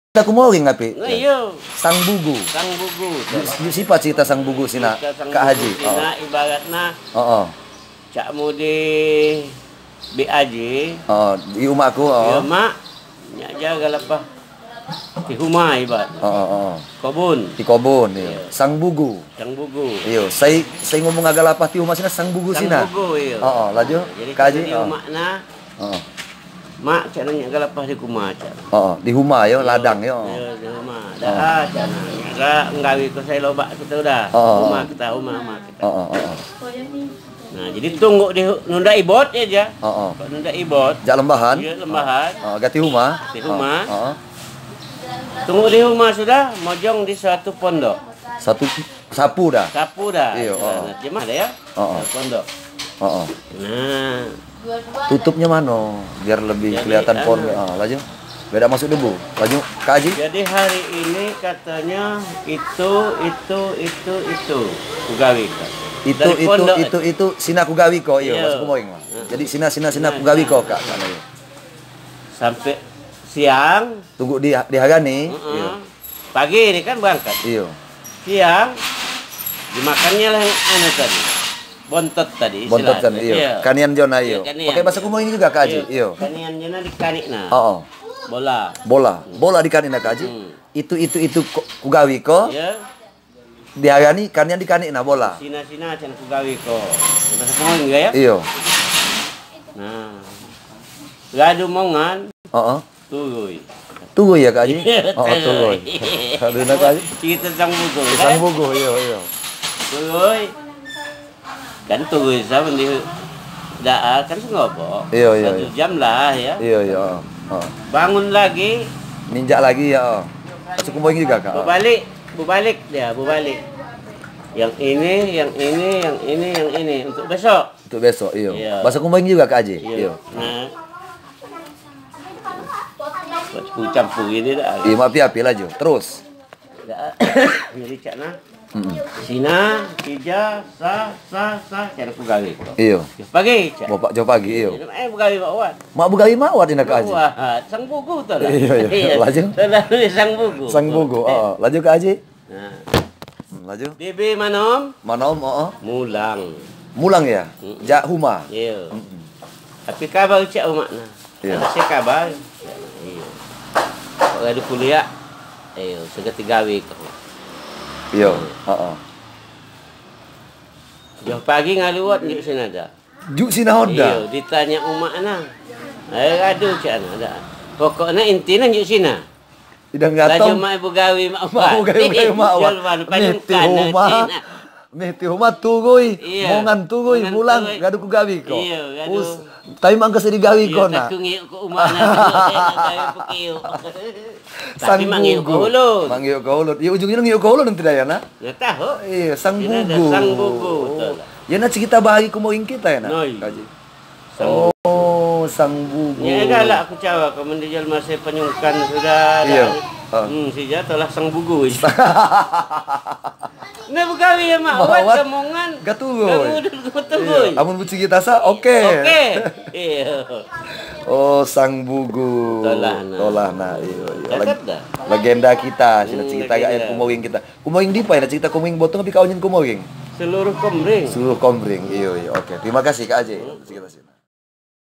#7 Cerita Komering " HIKAYAT SANG BUGU "
Cerita atau Hikayat Sang BUGU adalah cerita lama Komering yang bersipat Edukasi, semoga bermanfaat, terhibur, dan menginspirasi kita semua.. amin